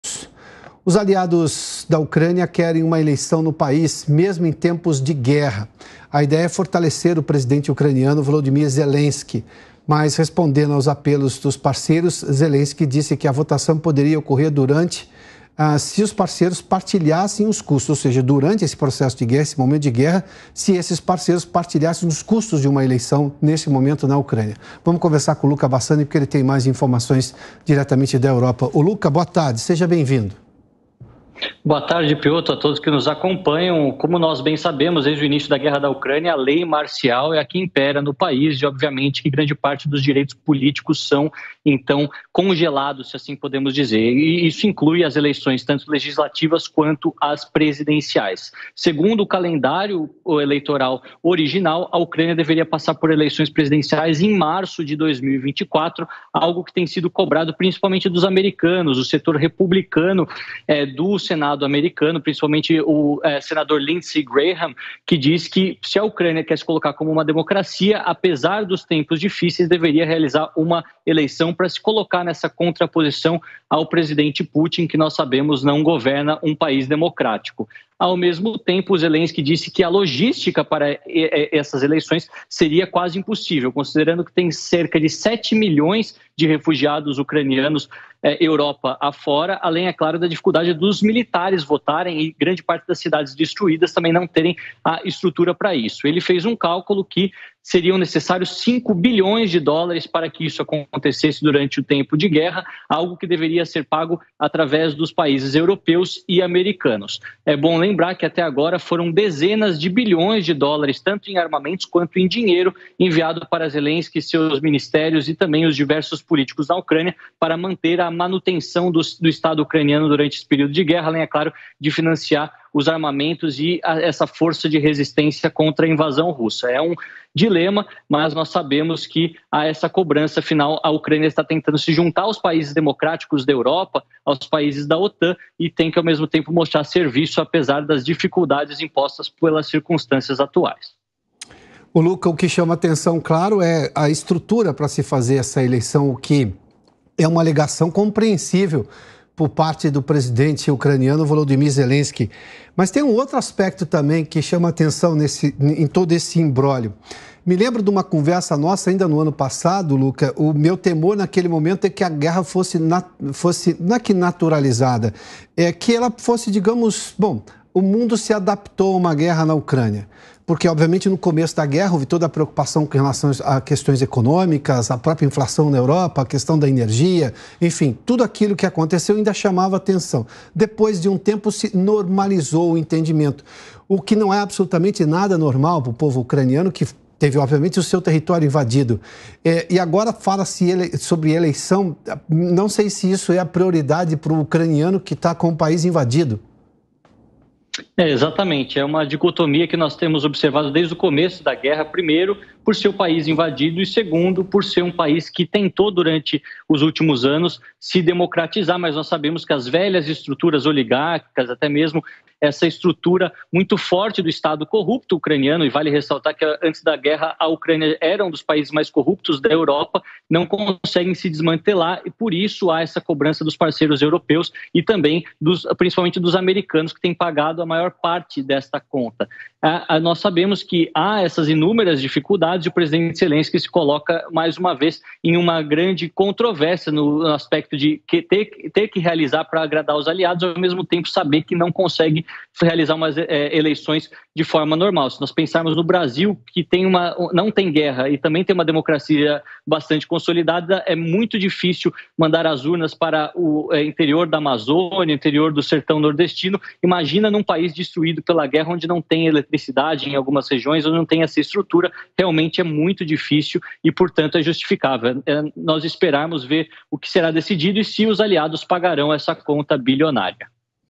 Zelensky quer que aliados arquem com custos da eleição presidencial na Ucrânia | PRÓS E CONTRAS
Os aliados da Ucrânia estão buscando a realização de novas eleições presidenciais, mesmo em meio ao contexto de guerra. Eles acreditam que tais eleições poderiam fortalecer a posição do atual presidente do país, Volodimir Zelensky. No entanto, o líder ucraniano afirmou que só concordará com a realização de novas eleições se os parceiros internacionais compartilharem os custos associados à organização do pleito.